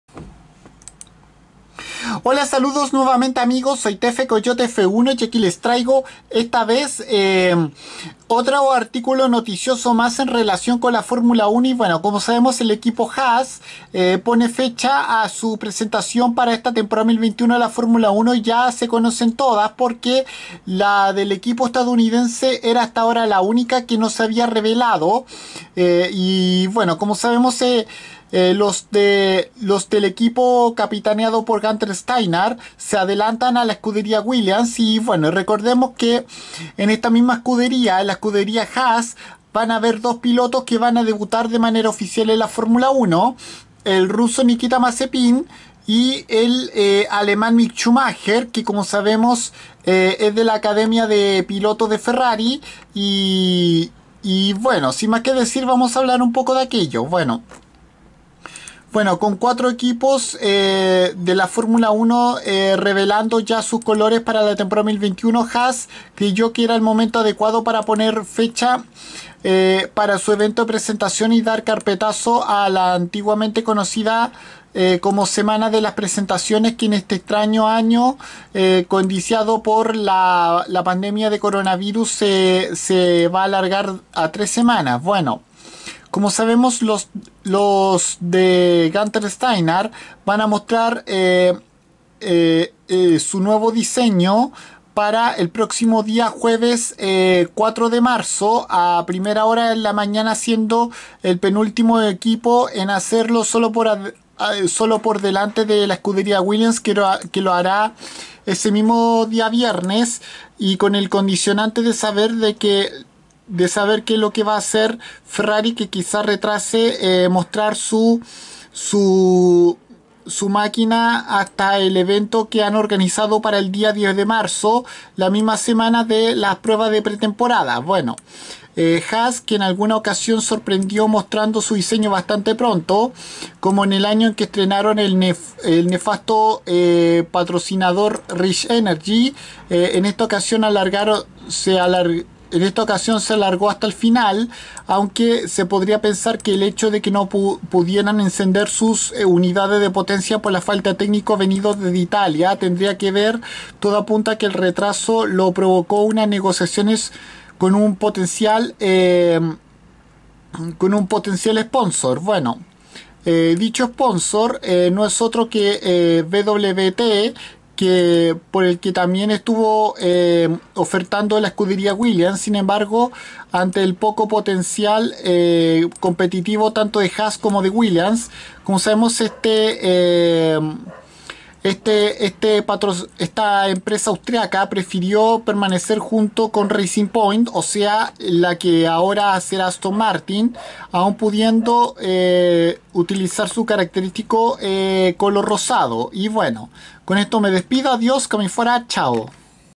Haas F1 Team presentará su nueva máquina para Temporada 2021 de Fórmula 1 el 4 de Marzo
Se trata del equipo estadounidense en el que debutarán en la categoría reina del automovilismo mundial tanto el ruso Nikita Mazepin como el joven alemán Mick Schumacher